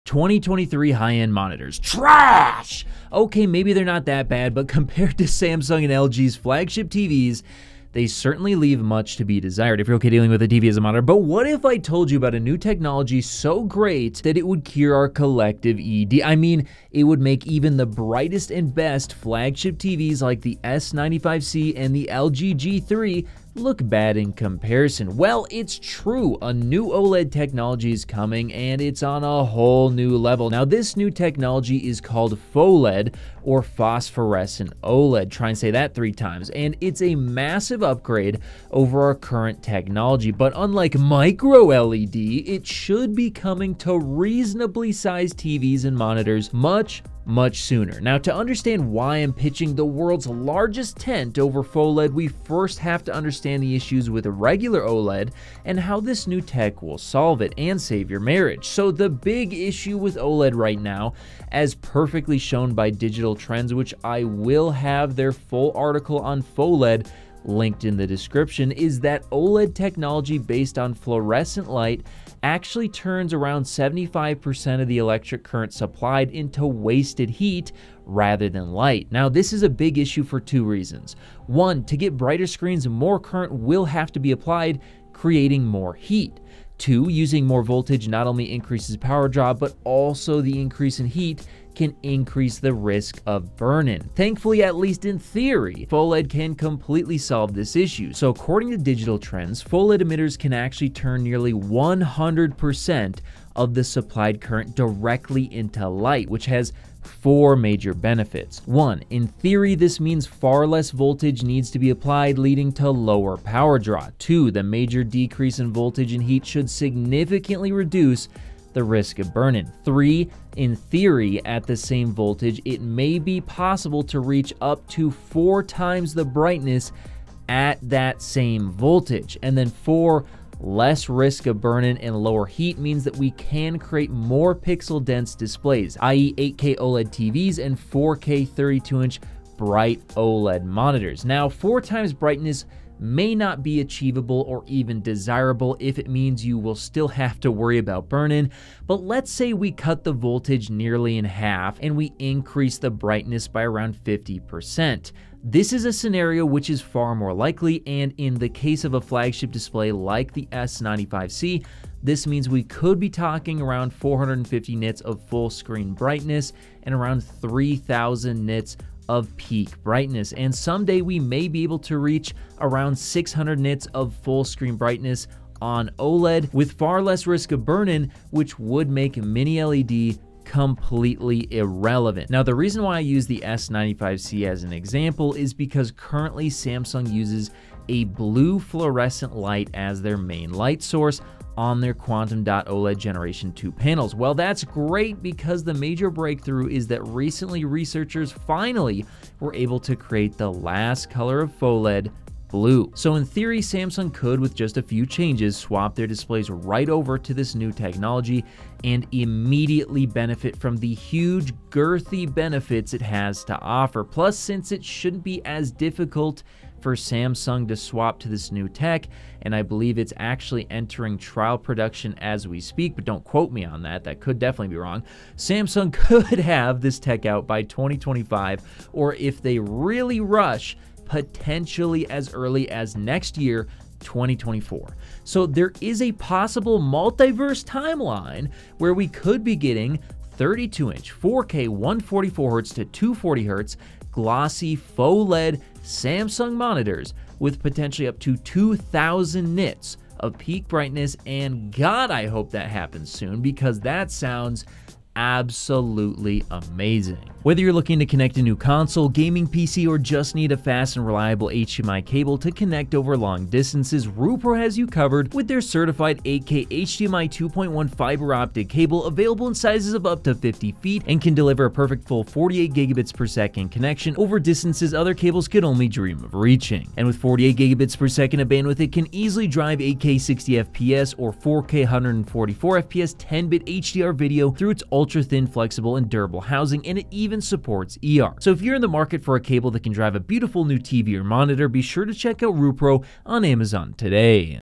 End Game - 32” 4K 240Hz Glossy QD PHOLED Monitor
2023 high end monitors… TRASH, ok maybe they're not that bad, but compared to Samsung and LG’s flagship TVs they certainly leave much to be desired (ifyourokdealingwithatvasamonitor), but what if I told you about a new technology so great that it would make even the brightest and best flagship TVs like the S95C and LG G3 look bad in comparison.

We’ll it's true. A new OLED technology is coming, and it's on a whole new level. Now this new technology is called PHOLED or Phosphorescent OLED, and it’s a massive upgrade over our current technology, but unlike Micro LED it should be coming to reasonably sized TVs and monitors much, much sooner.

So the big issue with OLED right now as perfectly shown by digital trends which I will have there full article on PHOLED linked in the description, is that OLED technology based on fluorescent light actually turns 75% of the electric current supplied into wasted heat rather than light. This is a big issue for 2 reasons.

To get brighter screens more current will have to be applied creating more heat.

Using more voltage not only increases power draw, but also the increase in heat can increase risk of burn in.

Thankfully at least in theory PHOLED can completely solve this issue, so according to digital trends PHOLED emitters can actually turn nearly 100% of the supplied current directly into light which has 4 major benefits.

In theory this means far less voltage needs to be applied leading to lower power draw.

The major decrease in voltage and heat should significantly reduce the risk of burn in.

In theory at the same voltage it may be possible to reach up to 4x the brightness at the same voltage.
Less risk of burn in and lower heat means we can create more pixel dense displays. IE 8K TVs and 4K 32” bright OLED monitors.

Now 4x brightness may not be achievable or even desirable if it means you will still have to worry about burn in, but let’s say we cut the voltage nearly in have and increase brightness by 50%. This is a scenario which is far more likely and in the case of a flagship display like the S95C this means that we could be talking about 450nits of full screen brightness, and around 3000 nits of peak brightness, and someday we may be able to reach 600nits full screen on OLED with far less burn in risk which would make mini LED completely irrelevant.

Now the reason why I used the S95C as an example is because currently Samsung uses a blue fluorescent light as their main light source on their QD OLED gen 2 panels. Well that’s great, because the major breakthrough is that recently researchers finally were able to create the last color of PHOLED blue.

So in theory Samsung could with few changes swap their displays right over to this new technology and immediately benefit from the huge, girthy benefits it has to offer. Plus since it shouldn’t be as difficult for Samsung to swap to this new tech and I believe its entering trial production as we speak, Samsung could have this tech out by 2025, or if they really rush potentially as early as next year 2024.

So there is a possible multiverse timeline where we could be getting 32” 4K 144Hz - 240Hz glossy PHOLED Samsung monitors with potentially up to 2000nits of peak brightness, and god I hope it happens soon, because that sounds absolutely amazing.